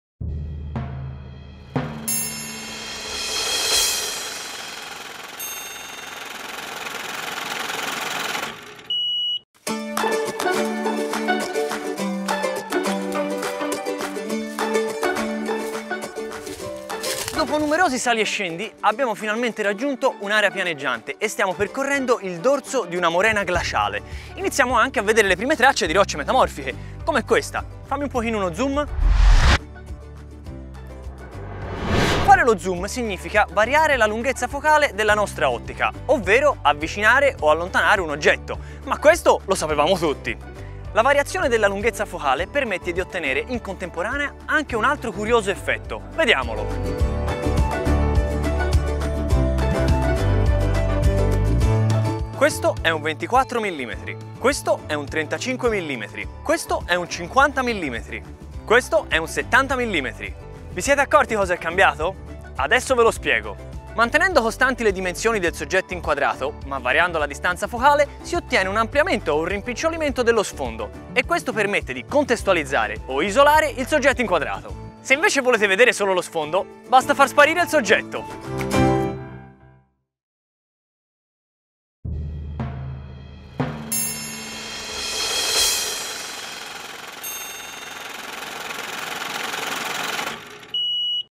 04. LUNGHEZZA FOCALE: lo sfondo; FOCAL LENGTH: the background
Sappiamo tutti che la lunghezza focale è quel numero in millimetri che indica se l'ottica è un grandangolo o un teleobiettivo (tecnicamente è la distanza tra la superficie del sensore e il centro della lente dell'obiettivo con il fuoco impostato all'infinito).
Ma sapevate quale altro fattore varia cambiando la lunghezza focale? Guardate qua!

We all know that the focal length is that number in millimeters that indicates whether the lens is a wide-angle lens or a telephoto lens (technically it is the distance between the sensor surface and the center of the lens with the infinity focus).
But did you know which other factor varies by changing the focal length? Look at this!